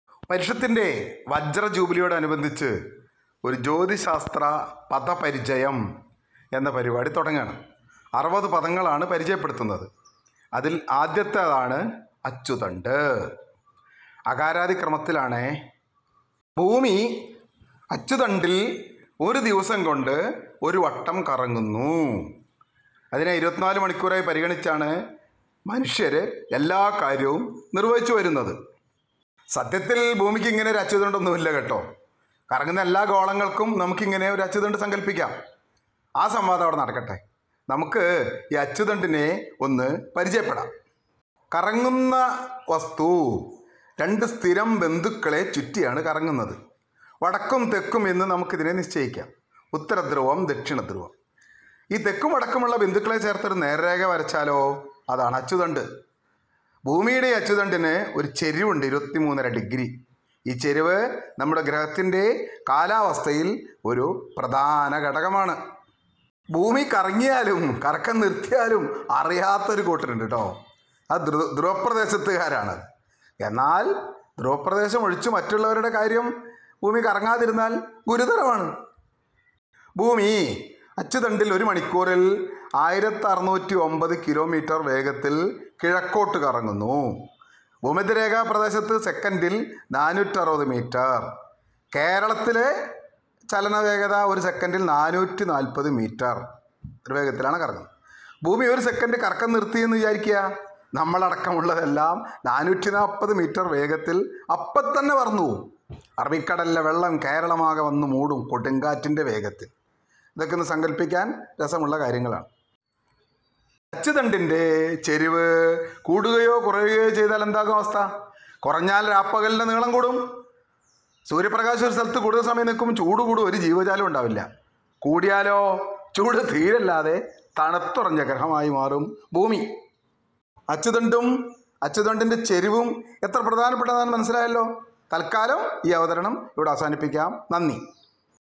അച്ചുതണ്ട് - ജ്യോതിശ്ശാസ്ത്ര പദാവലി 1
2021 -കേരള ശാസ്ത്രസാഹിത്യ പരിഷത്ത് അറുപതാം വാർഷികം ആഘോഷിക്കുന്ന വേളയിൽ  പരിഷത്ത് പ്രവർത്തകനായ എ.ശ്രീധരൻ  ജ്യോതിശാസ്ത്രത്തിലെ അറുപത് പദങ്ങളെ അകാരാദി ക്രമത്തിൽ പരിചയപ്പെടുത്തുന്നു...